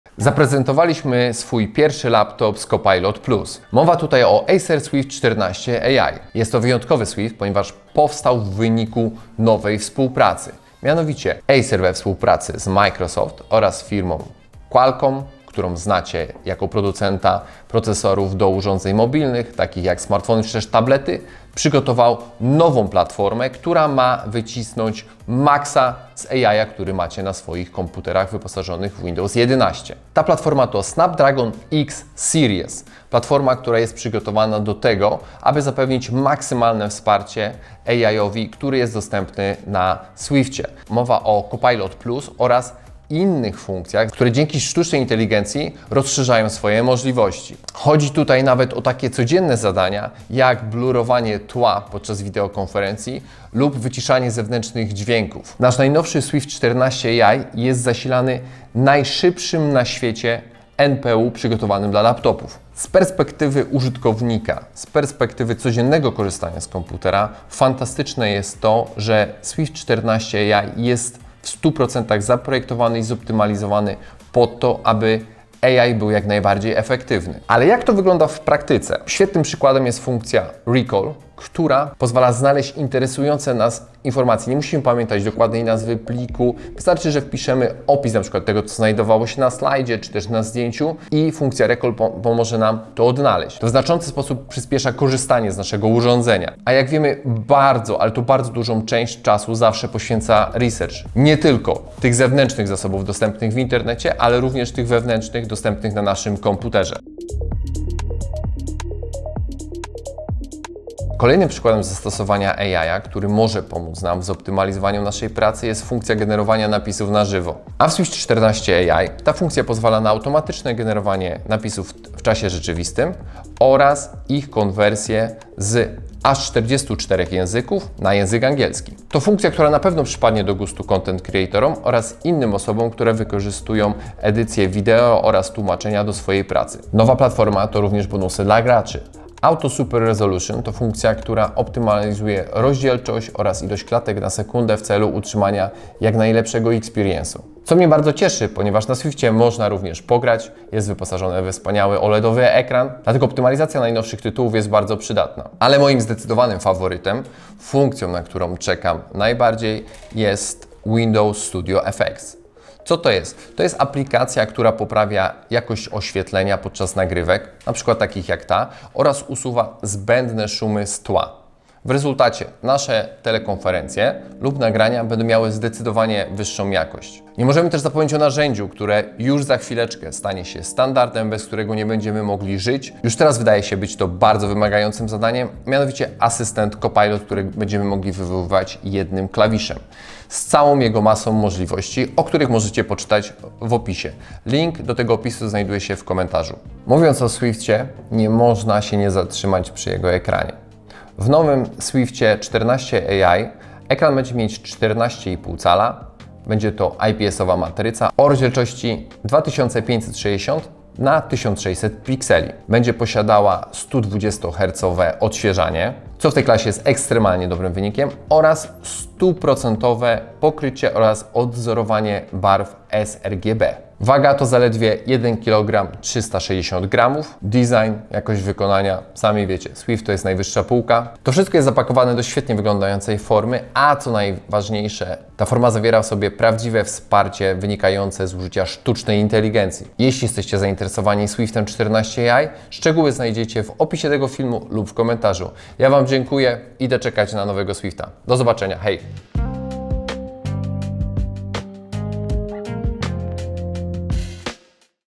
Acer Swift wchodzi w świat AI!
Jeremi Rybak, Szef Marketingu Acer w Polsce zaprezentuje Wam naszego laptopa wyposażonego w AI - Acer Swift 14 AI. Z filmu dowiecie się jakie możliwości otwiera przed Wami sztuczna inteligencja w laptopach Acer oraz jak może ułatwić codzienne zadania.

0:00 Acer Swift 14 AI.
0:30 Nowy procesor w naszym laptopie.
0:45 W czym pomaga AI?
0:55 Kilka słów o procesorze
1:19 Jak AI wygląda w praktyce?
2:35 Ulubiona funkcja Jeremiego.
3:18 Ekran robi robotę!
3:45 Kilka danych technicznych.